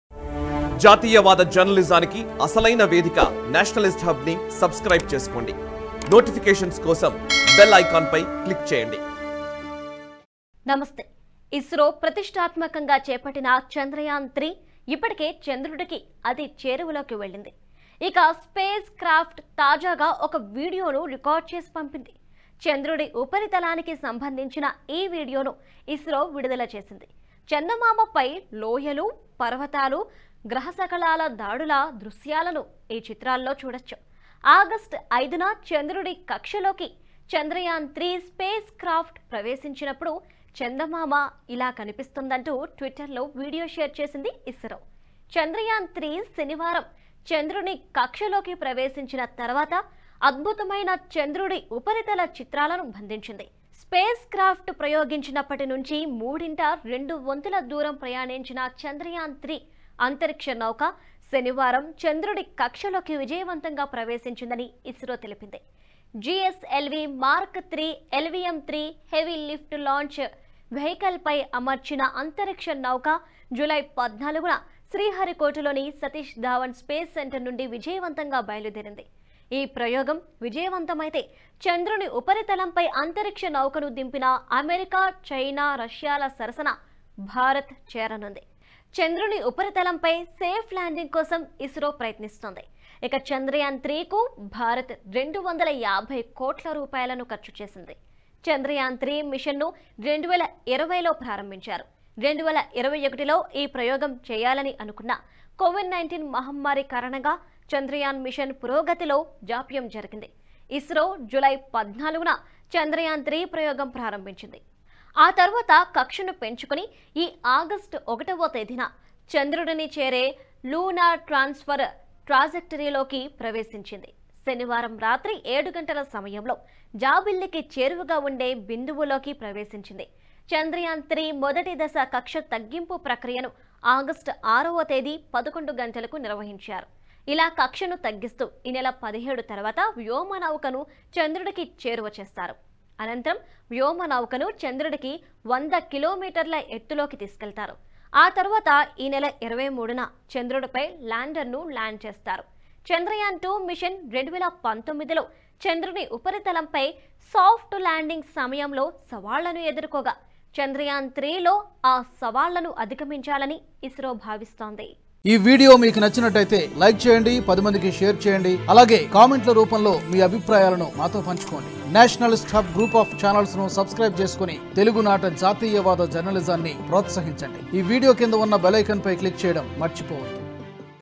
ISRO Shares First Images Of Moon Captured By Chandrayaan-3 | Lunar Orbit | Nationalist Hub
Our Company account details :
IFSC Code: HDFC0008045